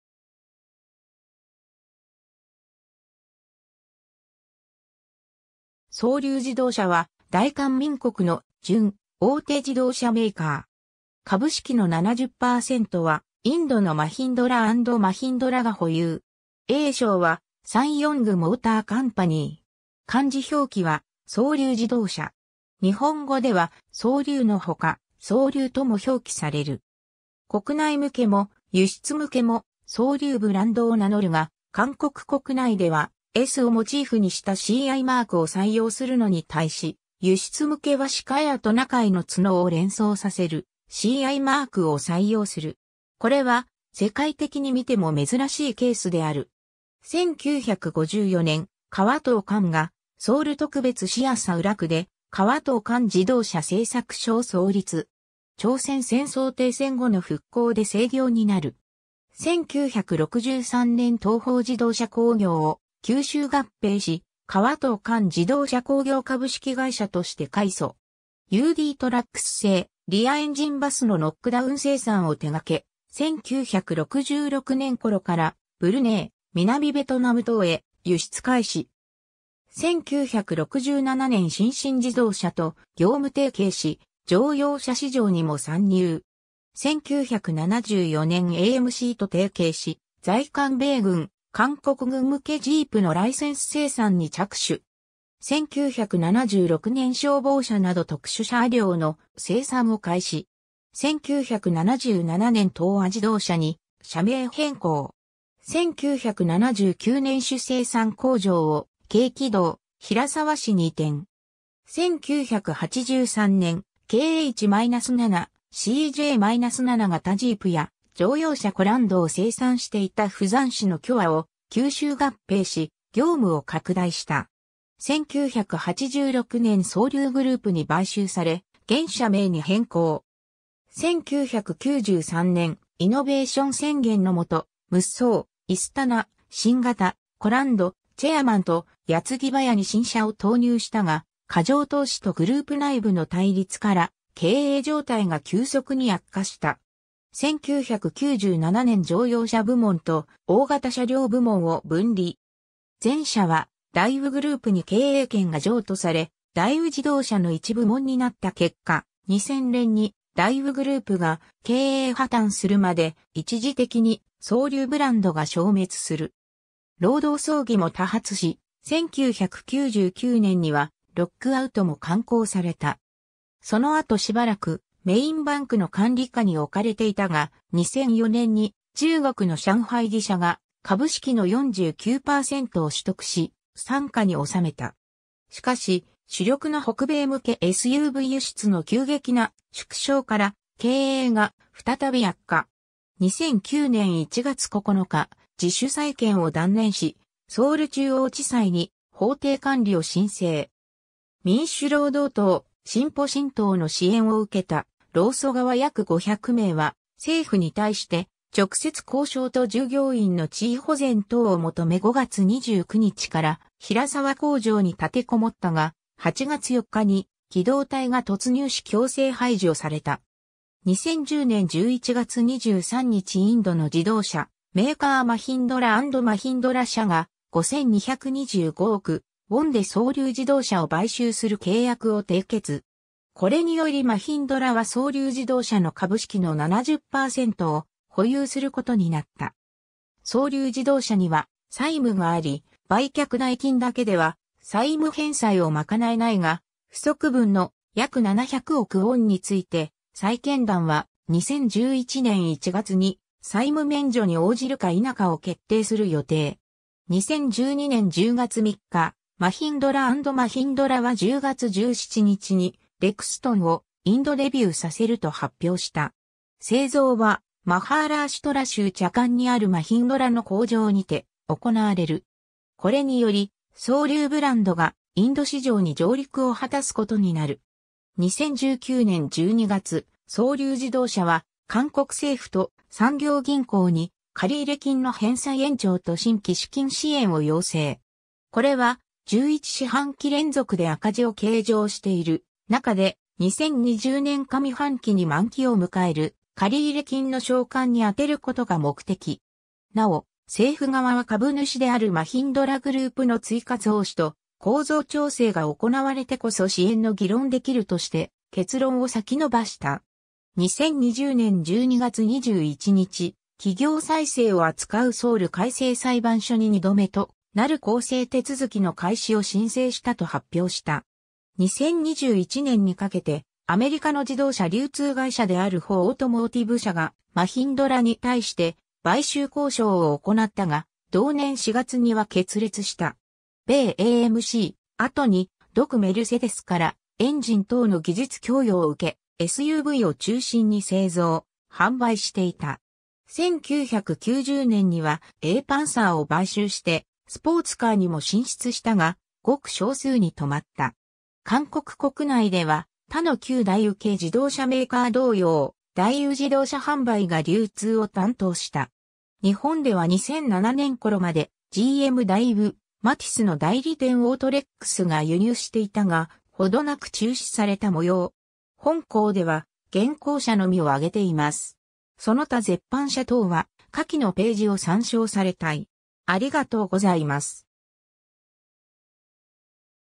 双竜自動車
韓国の自動車メーカー・ブランド
双竜自動車（サンヨンじどうしゃ）は大韓民国の準大手自動車メーカー。
株式の70%はインドのマヒンドラ&マヒンドラが保有。
英称はSsangYong Motor Company。
 漢字表記は「雙龍自動車」（「雙」は日本語の「双」に相当）。
日本語では「雙龍」の他、「双龍」とも表記される。
 国内向けも輸出向けも「雙龍」ブランドを名乗るが、韓国国内では「S」をモチーフにしたCIマークを採用するのに対し、輸出向けは鹿やトナカイの角を連想させるCIマークを採用する。
これは、世界的に見ても珍しいケースである。
 1954年　河東煥（ハ・ドンファン、하동환、1930年7月29日 - ）が、ソウル特別市麻浦区で『河東煥自動車製作所』（하동환자동차제작소、トラック・バス架装工場）を創立。
朝鮮戦争停戦後の復興で盛業になる。
 1963年　東方自動車工業（동방자동차공업）を吸収合併し、河東煥自動車工業株式会社として改組。
UDトラックス（旧:日産ディーゼル）製リアエンジンバスのノックダウン生産を手掛け、1966年頃からブルネイ、南ベトナム等へ輸出開始。
 1967年　新進自動車（신진자동차、後のGM大宇）と業務提携し、乗用車市場にも参入。
 1974年　AMCと提携し、在韓米軍・韓国軍向けジープのライセンス生産に着手。
 1976年　消防車など特殊車両の生産を開始。
 1977年　東亞自動車（동아자동차）に社名変更。
 1979年　主生産工場を京畿道平澤市に移転。
 1983年　KH-7, CJ-7 型ジープや、乗用車コランド（코란도）を生産していた釜山市の巨和（거화）を吸収合併し、業務を拡大した。
 1986年　雙龍グループに買収され、現社名に変更。
 1993年　『イノベーション宣言』の下、ムッソー（무쏘、1993年）、イスタナ（이스타나、1995年）、新型コランド（1996年）、チェアマン （체어맨、1997年）と、矢継ぎ早に新車を投入したが、過剰投資とグループ内部の対立から、経営状態が急速に悪化した。
 1997年　乗用車部門と大型車両部門を分離。
前者は大宇グループ（대우그룹）に経営権が譲渡され、大宇自動車の一部門になった結果、2000年に大宇グループが経営破綻するまで、一時的に雙龍ブランドが消滅する。
労働争議も多発し、1999年にはロックアウトも敢行された。
 その後しばらくメインバンクの管理下に置かれていたが、2004年に中国の上海汽車 (SAIC) が株式の49%を取得し、傘下に収めた。
しかし主力の北米向けSUV輸出の急激な縮小から、経営が再び悪化。
 2009年1月9日　自主再建を断念し、ソウル中央地裁に法定管理を申請。
民主労働党、進歩新党の支援を受けた労組側約500名は、政府に対して直接交渉と従業員の地位保全等を求め5月29日から平澤工場に立てこもったが、8月4日に機動隊が突入し強制排除された。
 2010年11月23日　インドの自動車メーカーマヒンドラ&マヒンドラ社が、5225億ウォンで雙龍自動車を買収する契約を締結。
これによりマヒンドラは雙龍自動車の株式の70%を保有することになった 。
雙龍自動車には債務があり、売却代金だけでは債務返済を賄えないが、不足分の約700億ウォンについて債権団は、2011年1月に債務免除に応じるか否かを決定する予定。
 2012年10月3日、マヒンドラ&マヒンドラは10月17日にレクストンをインドデビューさせると発表した。
製造はマハーラーシュトラ州チャカンにあるマヒンドラの工場にて行われる。
これにより、雙龍ブランドがインド市場に上陸を果たすことになる。
 2019年12月、双龍自動車は韓国政府と産業銀行に借入金の返済延長と新規資金支援を要請。
これは、11四半期連続で赤字を計上している中で、2020年上半期に満期を迎える借入金の償還に充てることが目的。
なお、政府側は株主であるマヒンドラグループの追加増資と構造調整が行われてこそ支援の議論できるとして結論を先延ばした。
 2020年12月21日、企業再生を扱うソウル回生裁判所に2度目となる更生手続きの開始を申請したと発表した。
2021年にかけて、アメリカの自動車流通会社であるHAAHオートモーティブ社がマヒンドラに対して買収交渉を行ったが、同年4月には決裂した。
 米AMC、後に独メルセデスからエンジン等の技術供与を受け、SUVを中心に製造・販売...